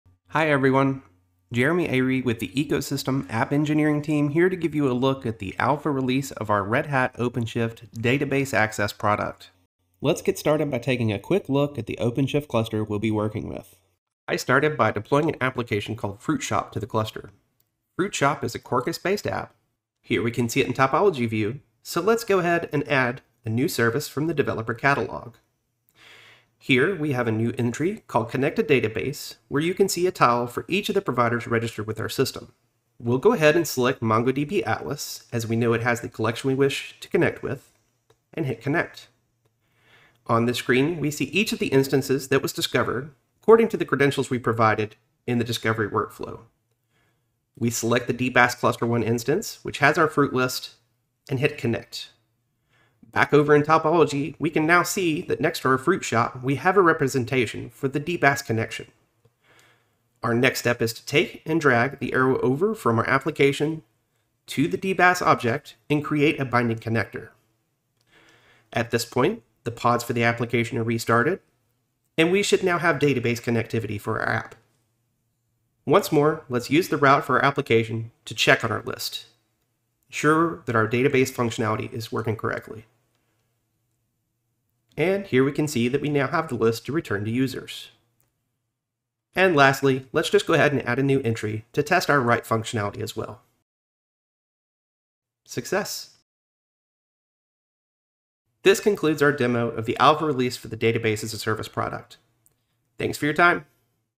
Developer preview demo of Red Hat OpenShift Database Access
See a preview demo of the early alpha release of Red Hat OpenShift Database Access, a planned cloud service offering on Red Hat OpenShift Dedicated or Red Hat OpenShift Service on AWS. Red Hat OpenShift Database Access makes it easier to discover, consume, monitor, and manage databases. In this demo, geared towards developers, Jeremy Ary runs through a demo of how a developer can easily connect to the database (MongoDB Atlas) and begin their work right away from within the OpenShift developer console.

If you do not see any connected databases, follow the steps in this video to create a provider account and assign a database to this cluster.